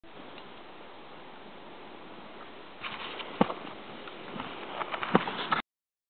Backyard Snowboarding 4
mark jumps a jump in his backyard